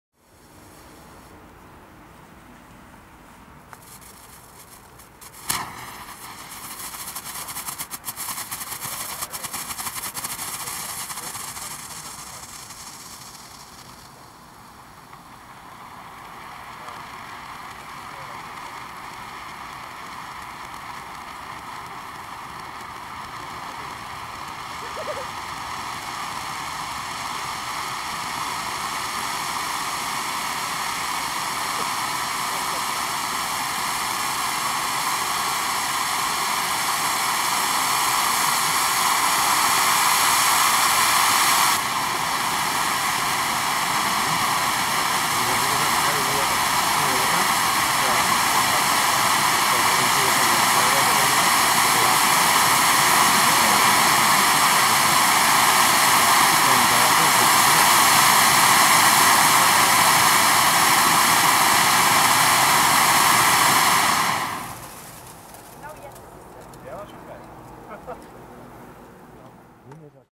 Bugano - Vulkan Magic Light - Kat 3 - 15.09.2016
Bei einem kleinem internen Vorschießen in Schleswig Holstein, wurden folgende Artikel vorgeführt:

Gaoo:
Elegia,
Senta,
All 22-01,
Purple Stars,
Flyffy Willow,
Happy Flower,
Amazing Night,
Diamond 3,
Range,
King Arthur,
Blossom,
I like it,
Rampage,
Alexander,
Obsession,
Exploder,
All 48-01

Xplode
Tower

Jorge:
Revolution 1,
Bombard JW901 (Kat 3)

Evoltion Fireworks;
Happy Thunder Q,

BlackBoxx (Kat) 3:
Crossette 1,
Crossette 2,
Colorful Taifun

Dann wurden noch 2 Diamond Red Star Bombenrohre, aus älterer Produktion (ca. 2009) getest.

Es wurde mit der Sony HDR CX130E gefilmt